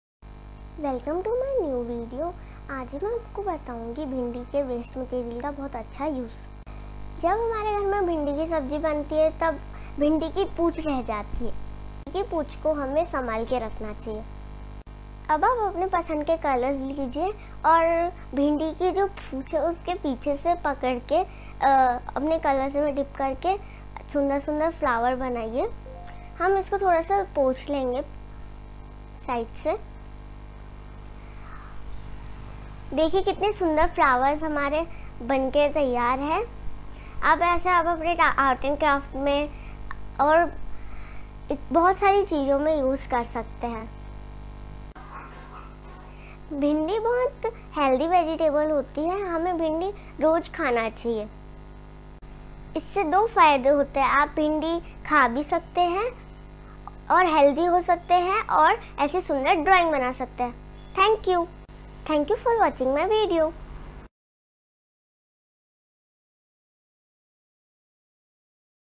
creative bhindi waste art
kids art
best use of waste
kids creative corner

healthy to eat and good use of art by vegetable lady finger "bhindi" !